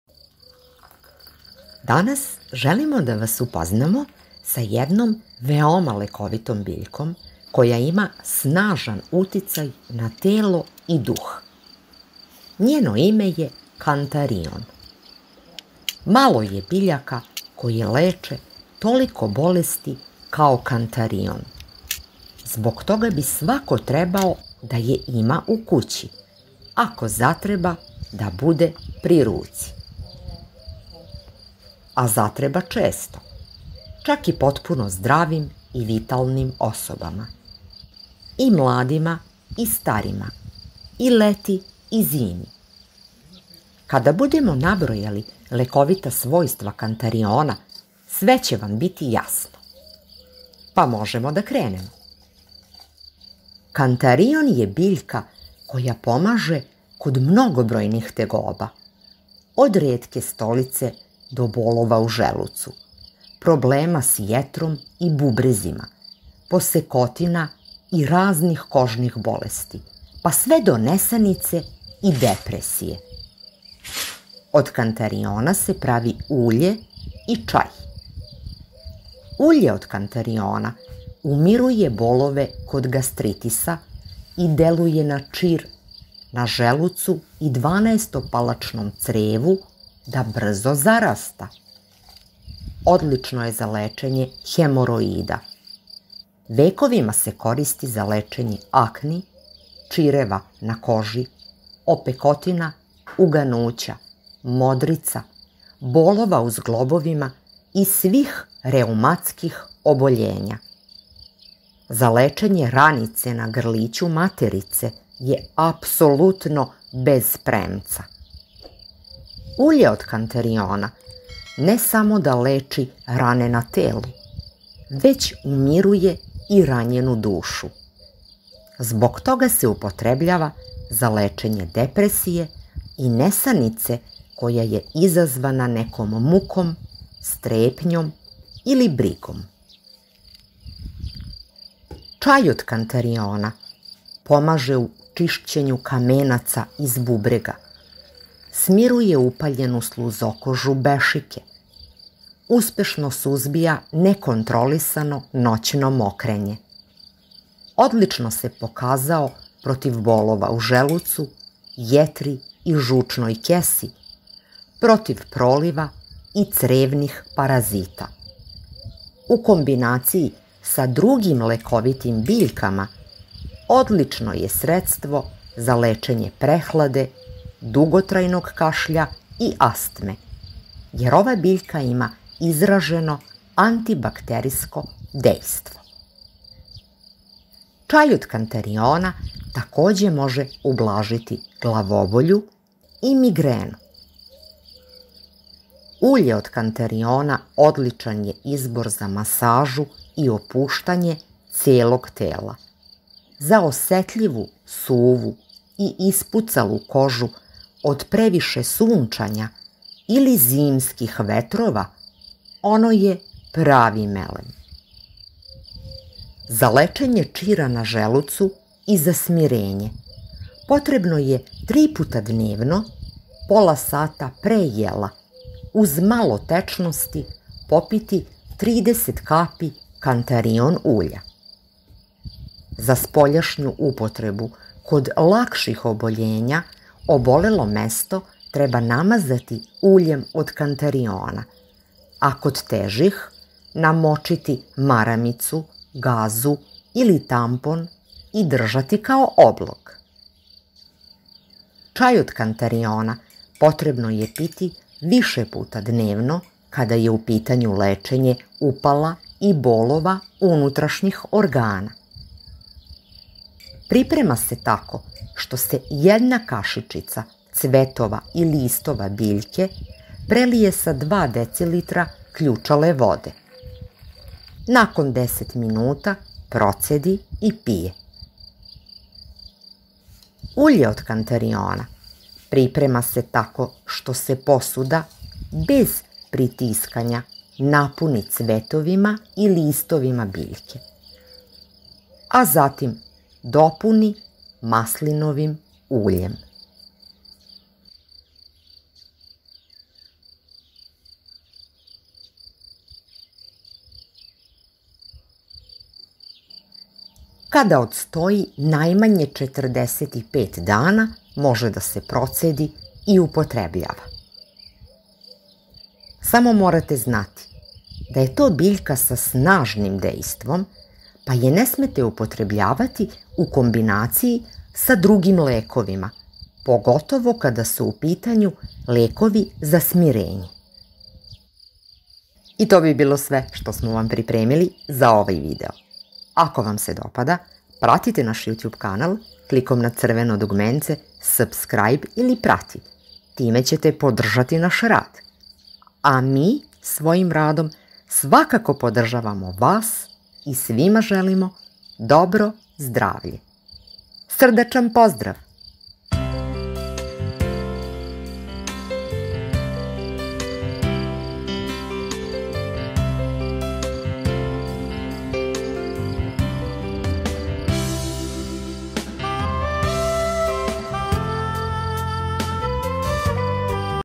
Zašto ovu biljku uvek treba imati u kući
Malo je biljaka koje leče toliko bolesti kao kantarion, jer ima snažan uticaj na telo i duh. Zbog toga bi svako trebao da je ima u kući, ako zatreba da bude pri ruci.

Time ćete podržati naš rad. A mi svojim radom svakako podržavamo vas i svima želimo dobro zdravlje.